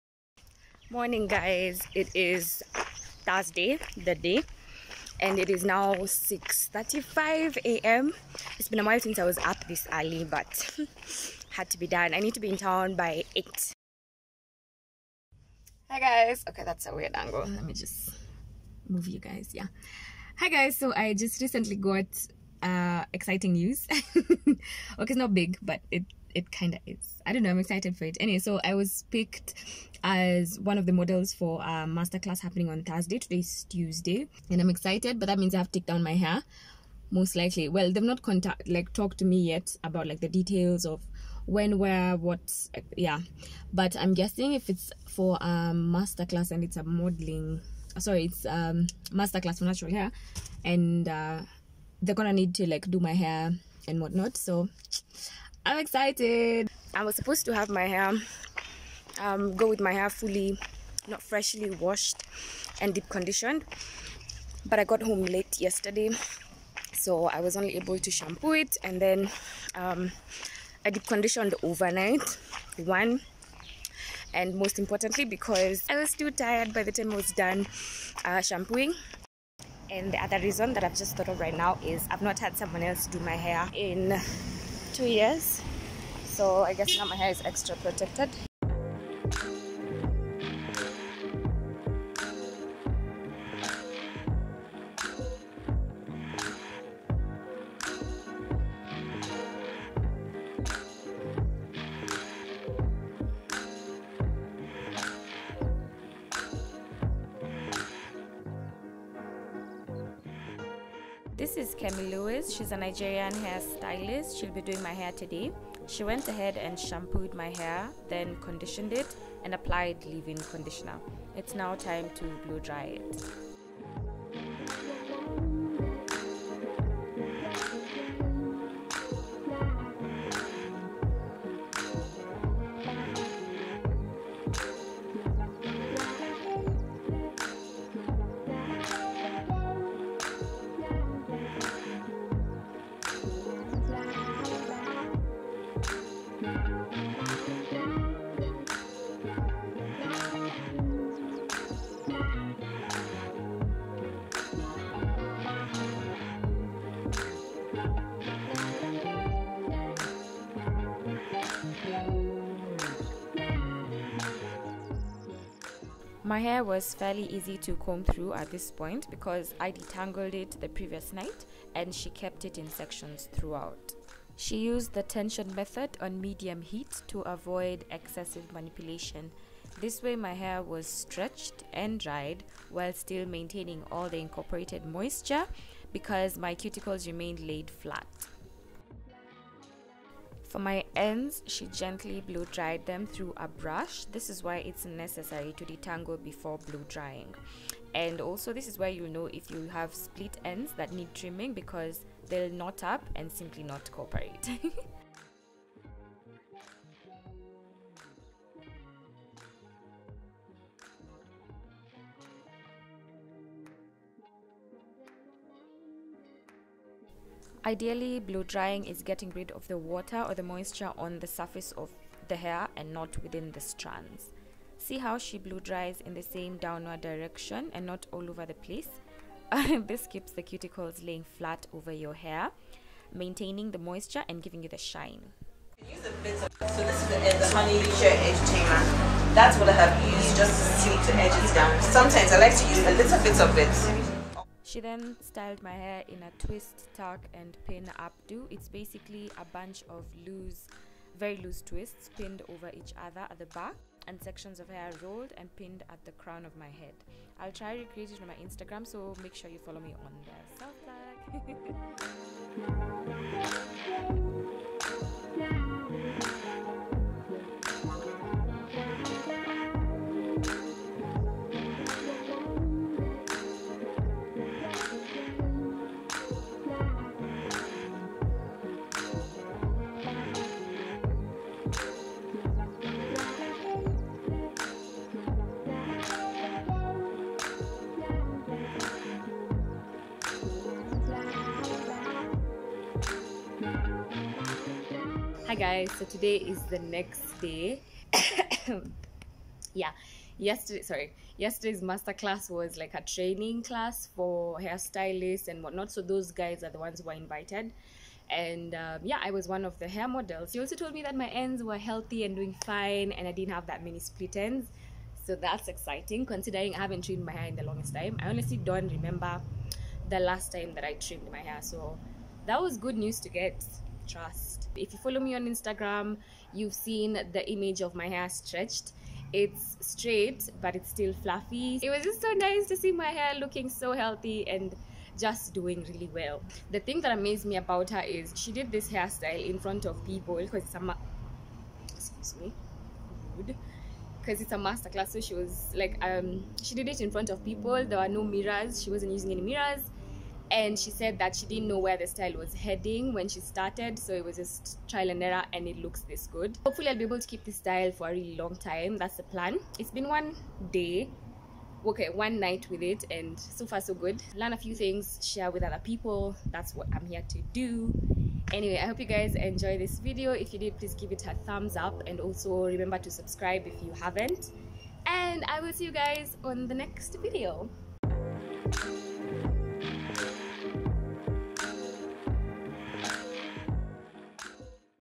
Salon Visit 2019 • Bonus Blow-drying Tips
Hey guys,
I recently got my hair done by a professional and this is how it went. Kemi Lewis, a Nigerian natural hair stylist, did my hair from shampoo, condition, blow-drying to styling. She also shared some tips with me and of course I share them with you.
This was during a Design Essentials Masterclass for hair stylists.

Third person view footage provided by Ashley's Hair and Beauty Academy.

Friends?